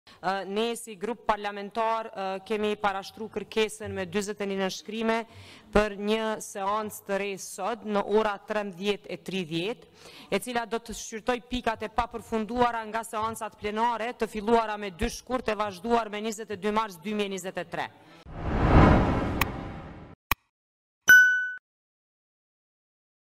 LVV kërkon seancë të re
Lëvizja Vetëvendosje përmes 41 nënshkrimeve ka kërkuar që sot në 13:30 të mbahet seancë e re për shqyrtim të pikave të papërfunduara.